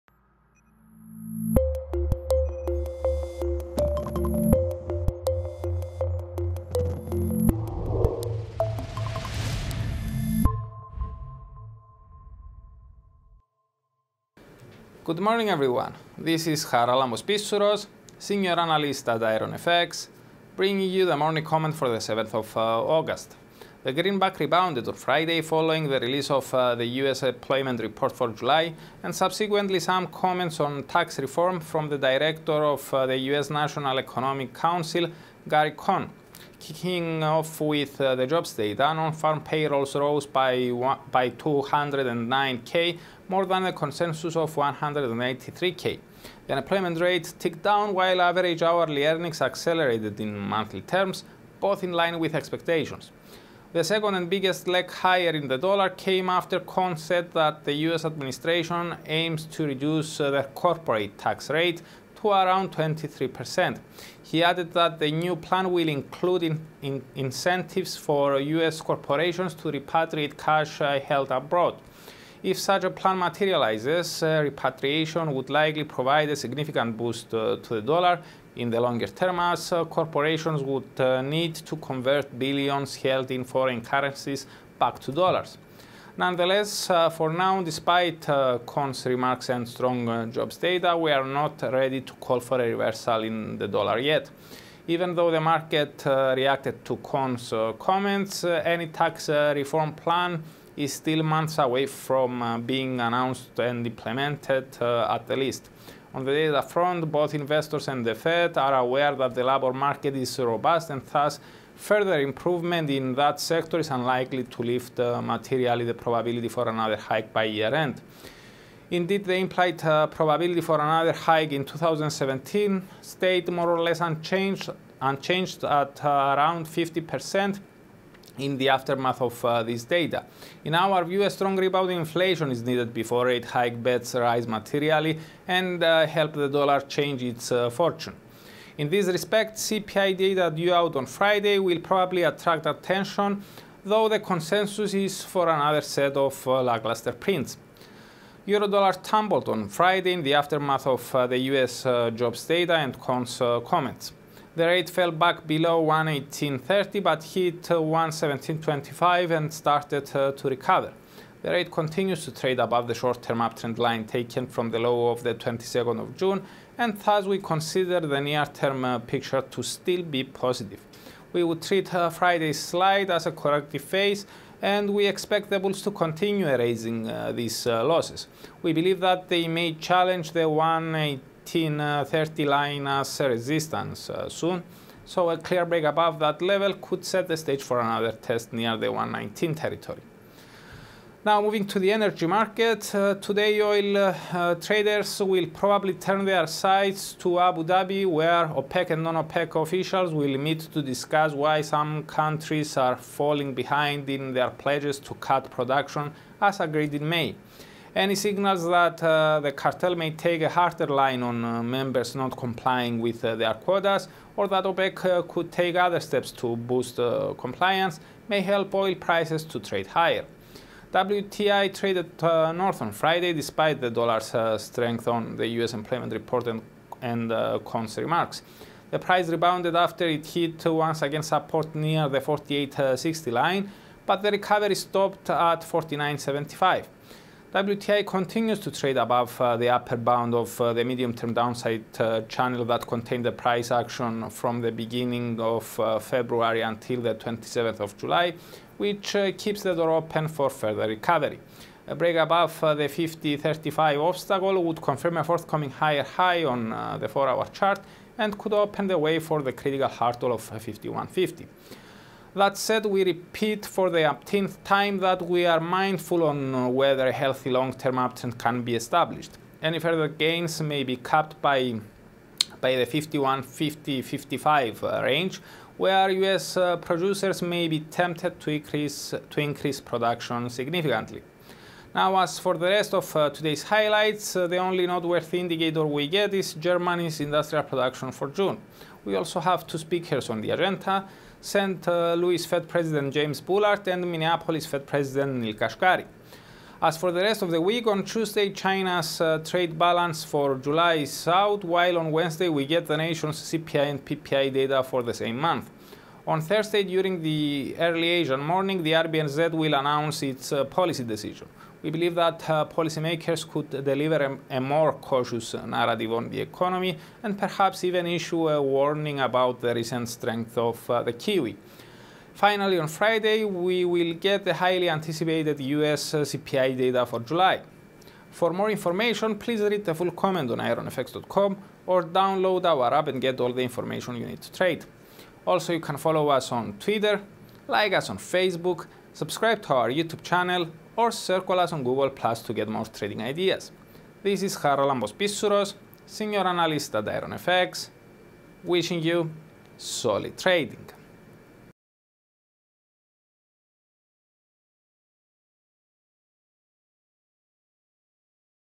IronFX Daily Commentary by Charalambos Pissouros | 07/08/2017
Strong jobs data and comments on tax reform lift the dollar
• USD rebounded on Friday, after the release of the July jobs report and some comments from Gary Cohn.
• The jobs data were strong overall. NFP came 209k, higher than the consensus of 183k, while the unemployment rate ticked down and earnings accelerated, as expected.
• The second leg higher in USD was fueled by Cohn saying the US aims to reduce corporate tax to below 23%.
• He also said the tax plan will include incentives for US corporations to repatriate cash held abroad.
• Despite the USD-positive reaction, we doubt these are a game-changer for USD. Focus is on Friday’s CPIs.

Oil producers meet to discuss compliance with production cuts
• OPEC and non-OPEC producers meet to discuss why some members aren’t cutting production as agreed.
• Investors will probably look for any signals that OPEC may take steps to boost compliance with the deal.
• Any such signals could lead to further rebound in WTI (but future gains may be limited near the $51.50 area).

Today’s highlights:
• Speakers: St. Louis Fed Pres. Bullard (15:45 GMT) and Minneapolis Fed Pres. Kashkari (17:25 GMT).

As for the rest of the week:
• Tue: China’s trade balance for July is due out.
• Wed: China’s CPI & PPI rates for July are both expected unchanged. We see the case for positive surprises.
• Thu: RBNZ policy gathering. The Bank is likely to remain on hold. Policymakers could deliver a more cautious narrative, and perhaps a warning about the recent strength of NZD.
• Fri: US CPI data for July. The consensus is for the headline rate to tick up and for the core to stay unchanged. Market focus will be primarily on any surprise in the core.